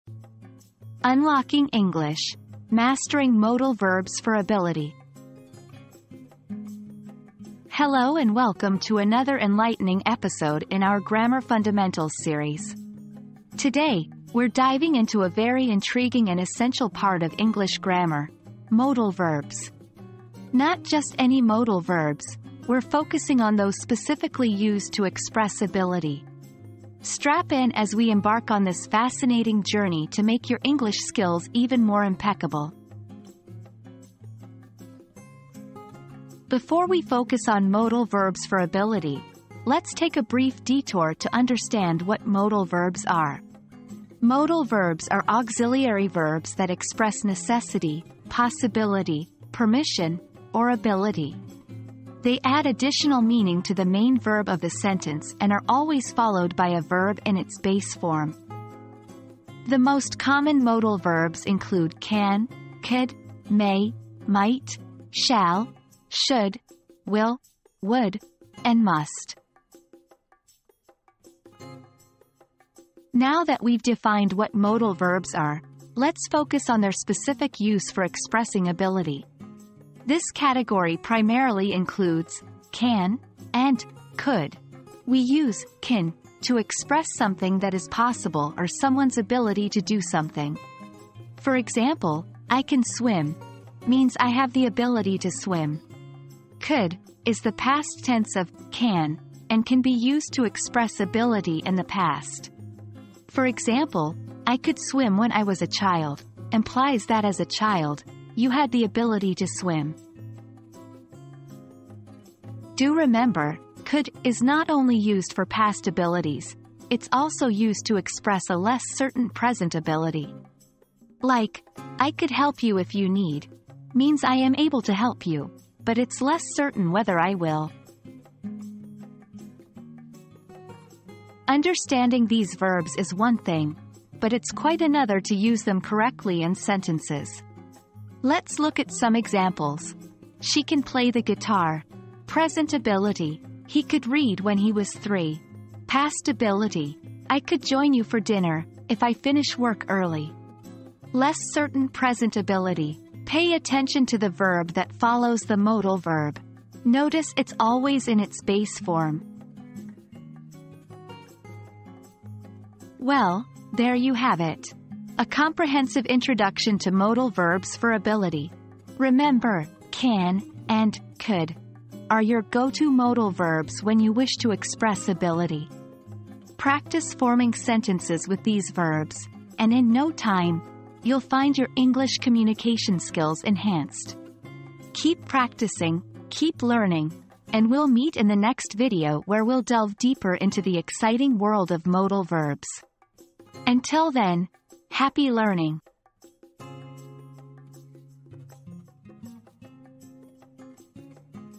Unlocking English: Mastering Modal Verbs for Ability
Mastering Modal Verbs for Ability: Unlock Your English Potential • Join us on a fascinating journey to understand and master modal verbs for ability in English grammar. Learn how to use 'can' and 'could' to express possibility and past or present ability. Improve your English skills and unlock your potential today!

00:00 • Introduction - Unlocking English: Mastering Modal Verbs for Ability
00:35 • Understanding Modal Verbs
01:12 • Modal Verbs for Ability
02:10 • Using Modal Verbs for Ability in Sentences